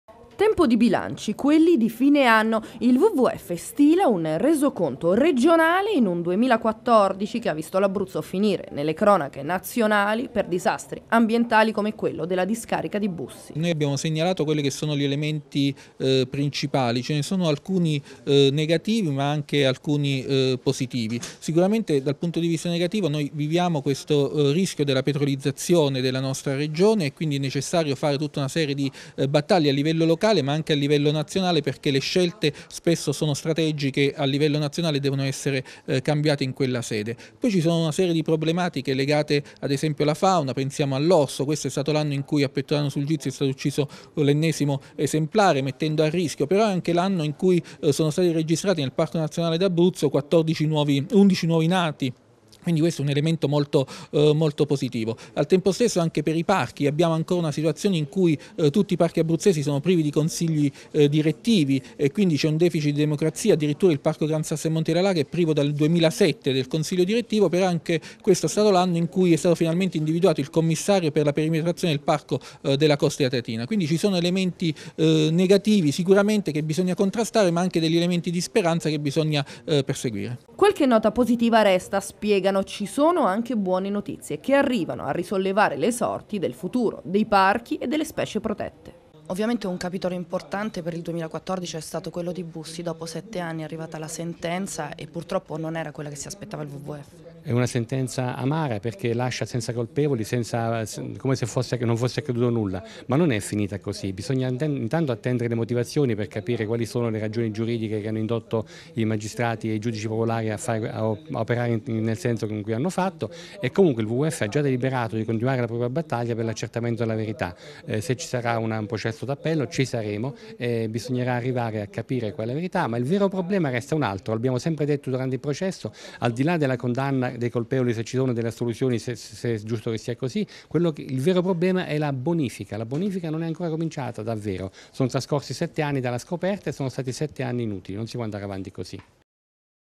Il 2014 dell'Ambiente d'Abruzzo
E' tempo di bilanci di fine anno anche per il WWF che, con Luciano di Tizio, traccia un consuntivo del 2014 che sta per terminare